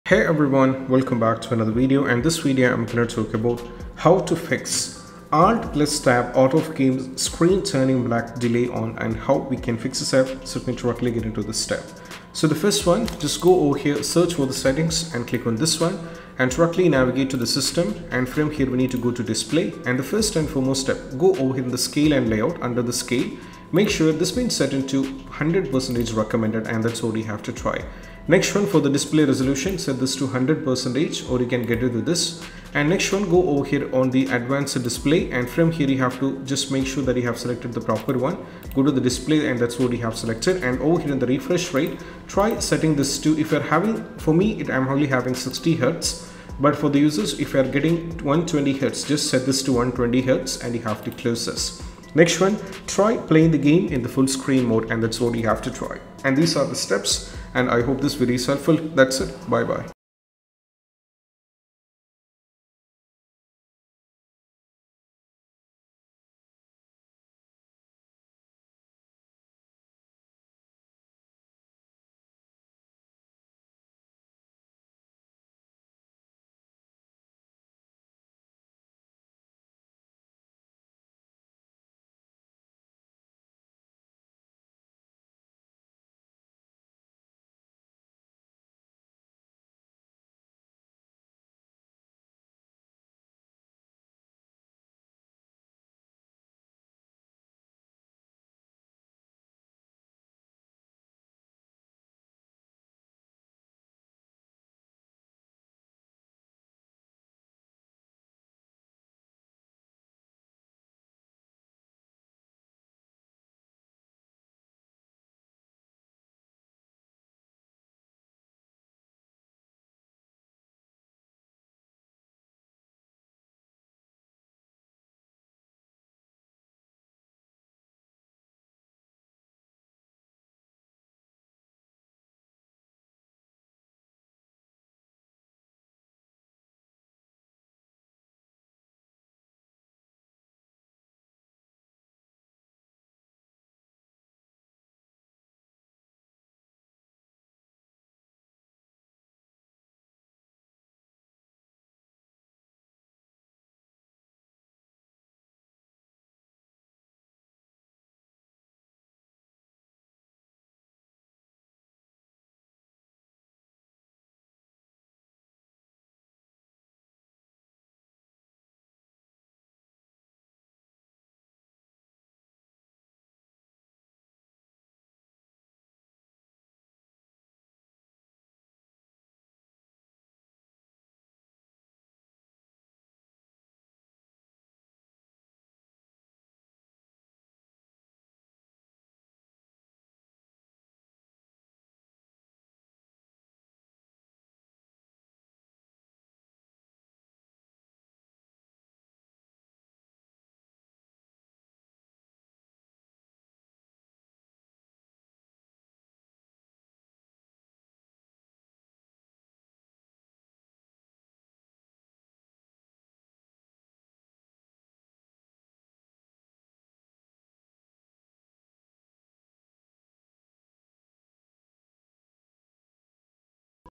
How to Fix Black Screen or Delay When Using Alt + Tab in Games
In this video, we’ll show you how to fix the black screen or delay issue when using Alt + Tab to switch out of games. If your screen goes black or lags while toggling between windows, don’t worry—this guide has the fixes you need. We’ll walk you through quick settings tweaks and easy solutions to make Alt + Tabbing smoother and more responsive, so you can switch apps without any interruptions.